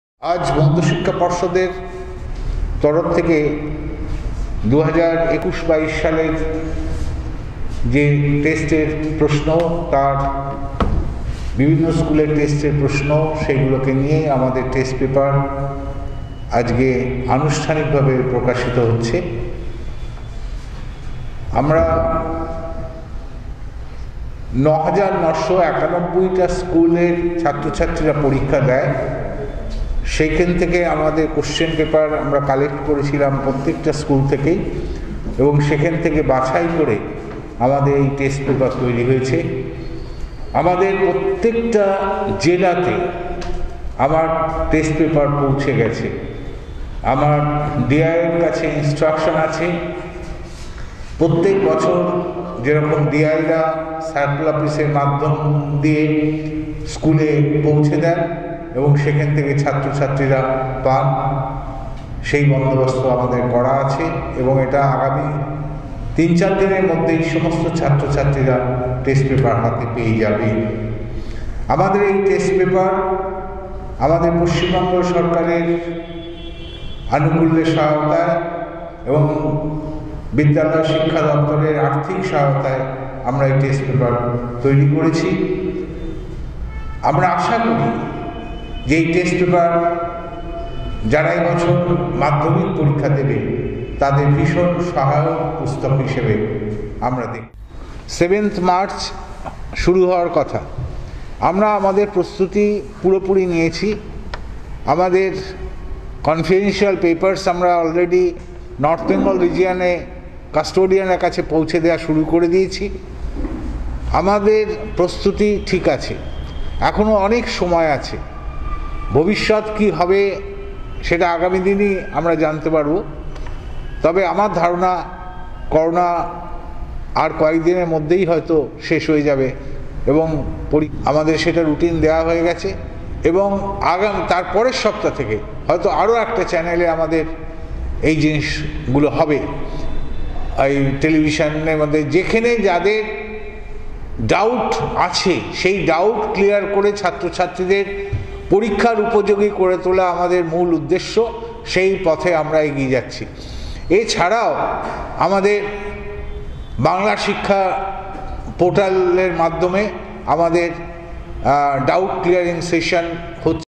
💥মাধ্যমিক পরীক্ষা নিয়ে মধ্যশিক্ষা পর্ষদের বক্তব্য।। WBBSE।।
মাধ্যমিক পরীক্ষা নিয়ে মধ্যশিক্ষা পর্ষদ কি বললেন আজকের ভিডিও তে সেটিই তুলে ধরা হয়েছে।